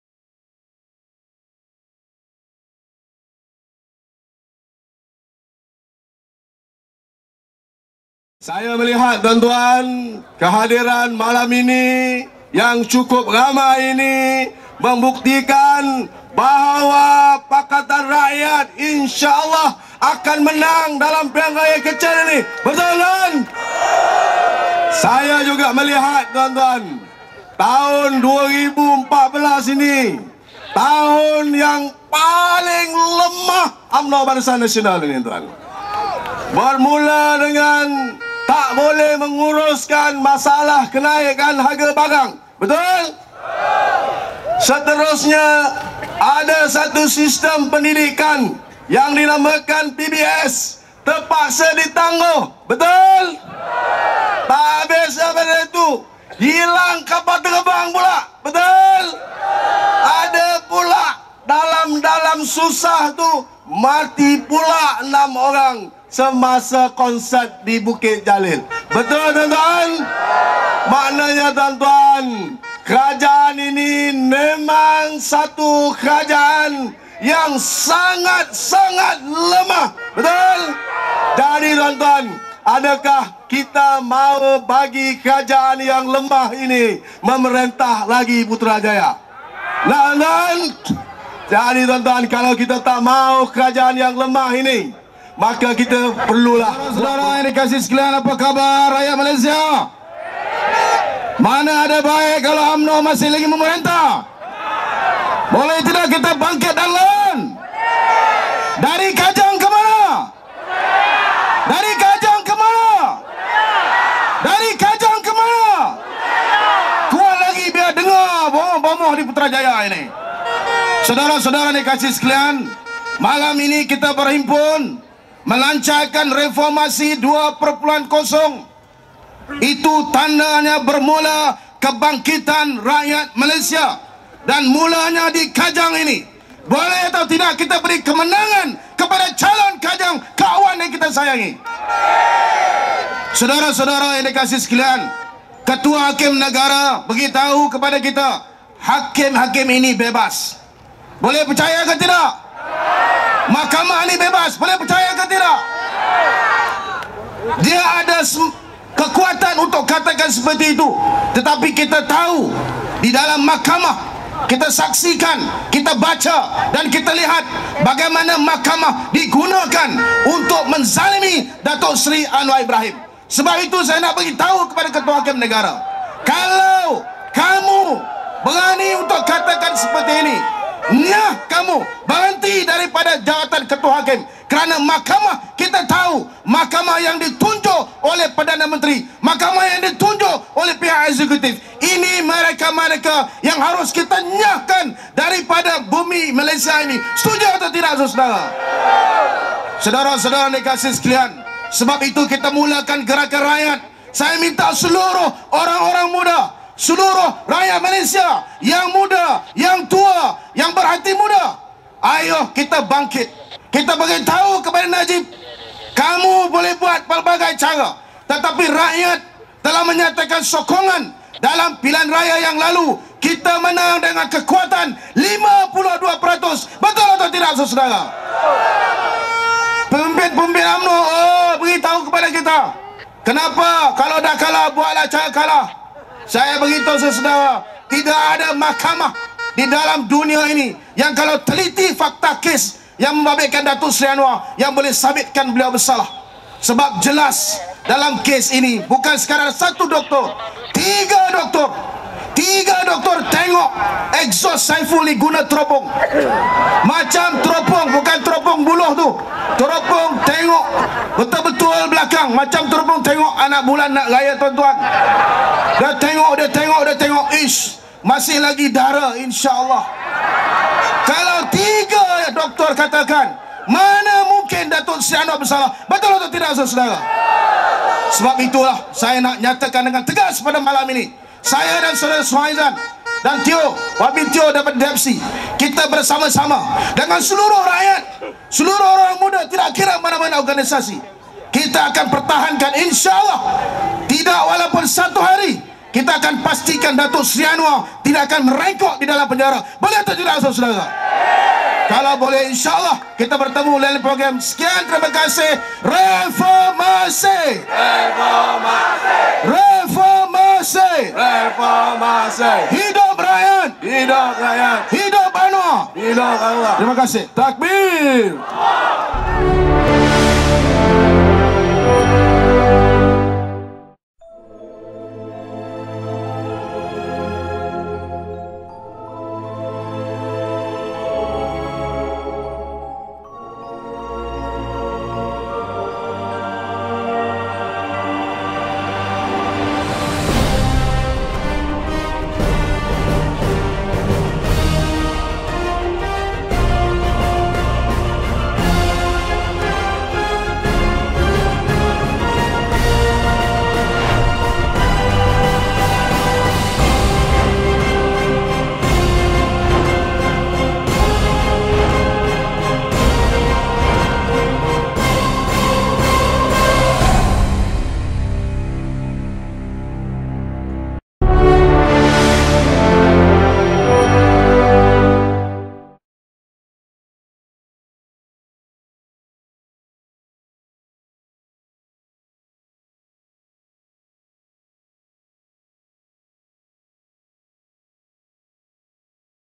Shamsul Iskandar: Seluruh Orang-Orang Muda, Seluruh Rakyat Malaysia, Ayuh Kita Bangkit
Himpunan Reformasi 2.0, Stadium Kajang 21/03/2014